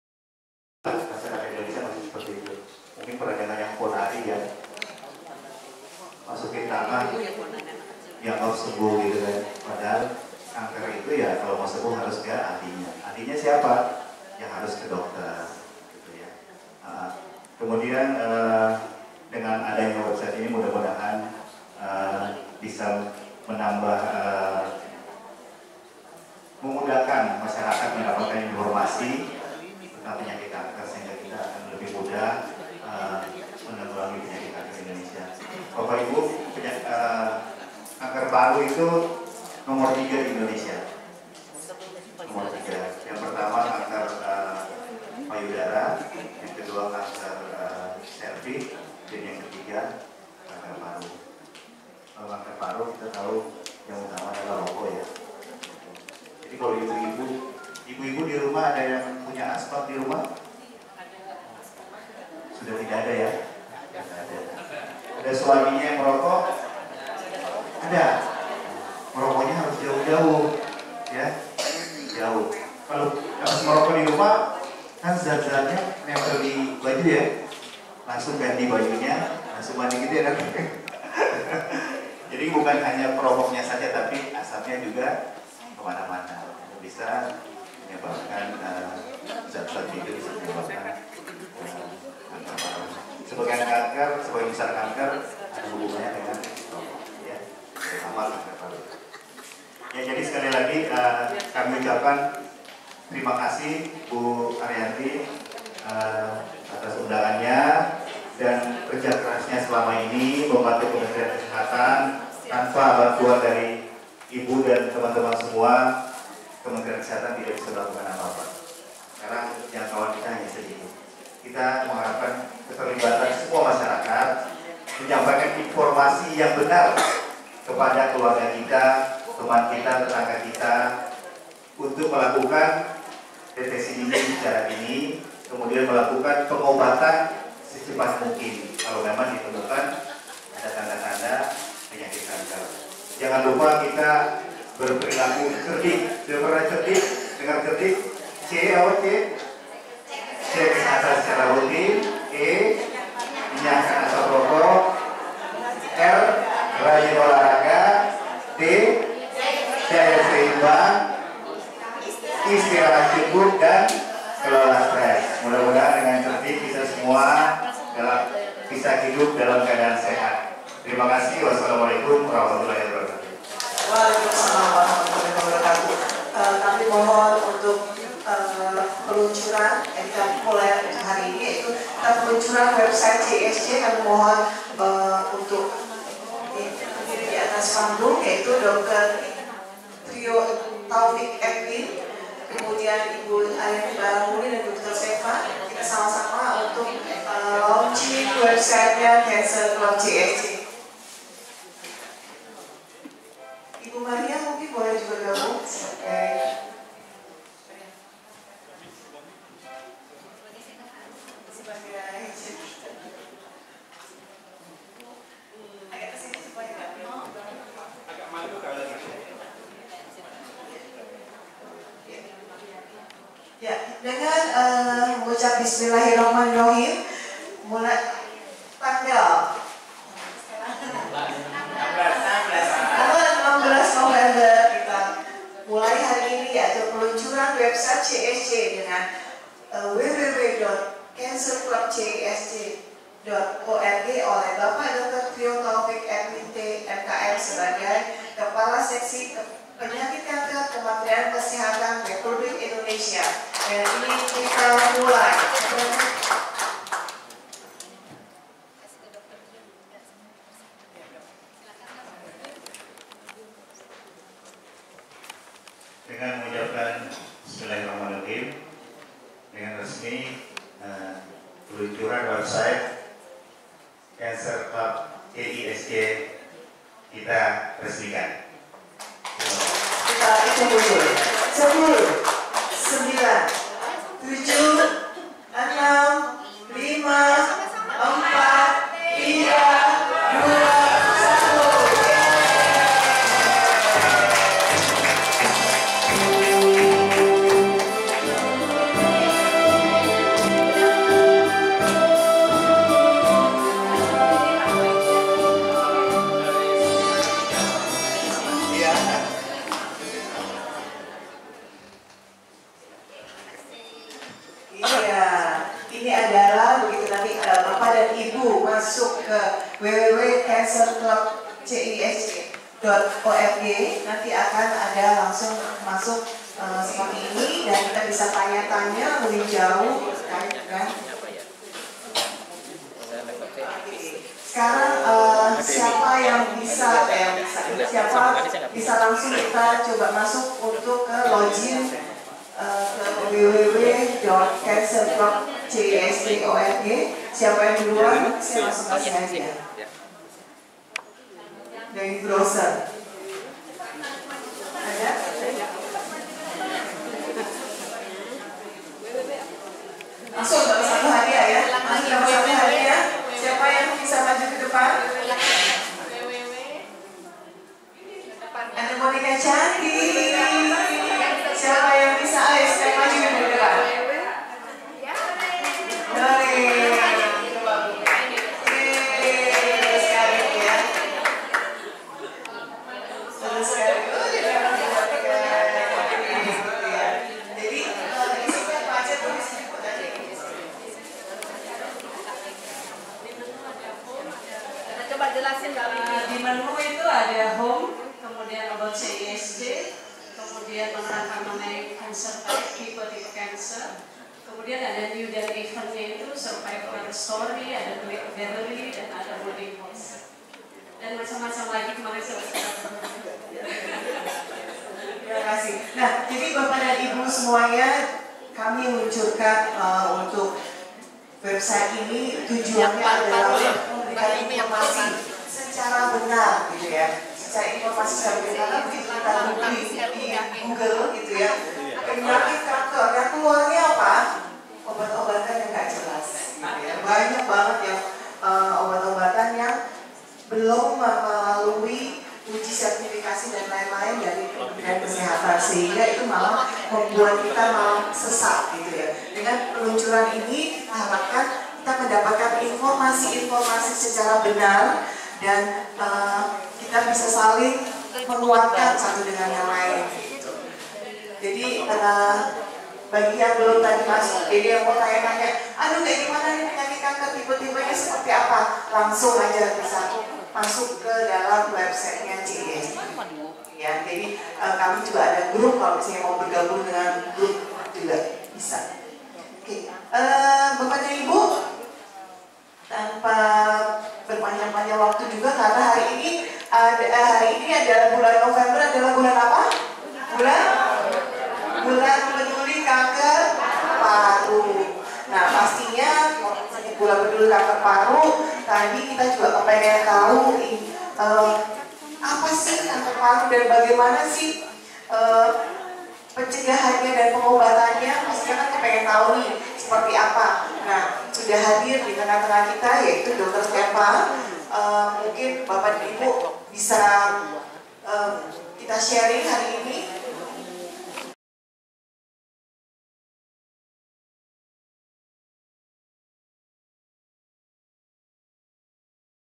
Peresmian CancerClubCISC org
Peluncuran sebagai media informasi tentang penanggulangan kanker di Indonesia. Semoga bermanfaat.